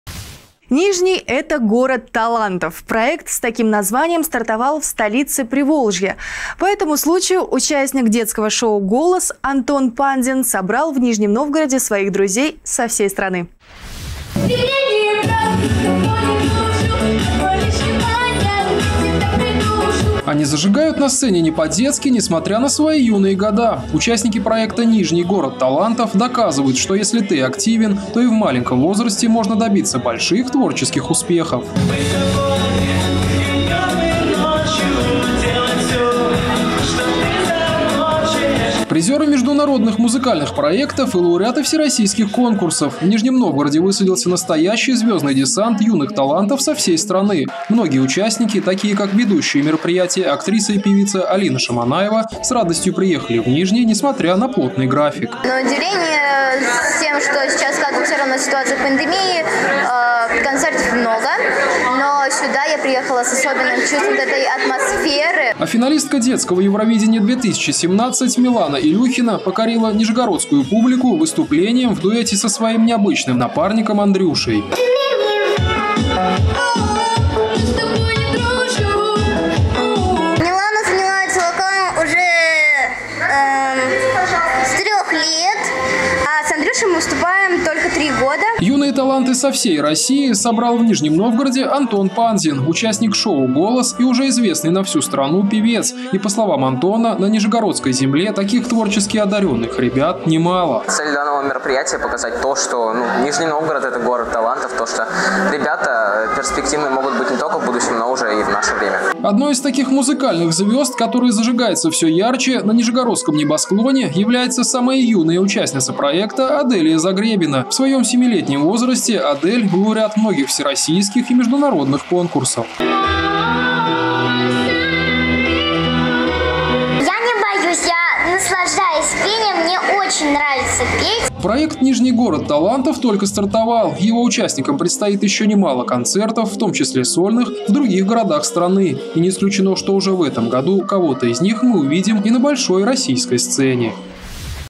Голоса Приволжья
Нижний - город талантов. Проект с таким названием стартовал в столице Приволжья. По этому случаю участник детского шоу Голос - Антон Панзин собрал в Нижнем Новгороде своих друзей со всей страны.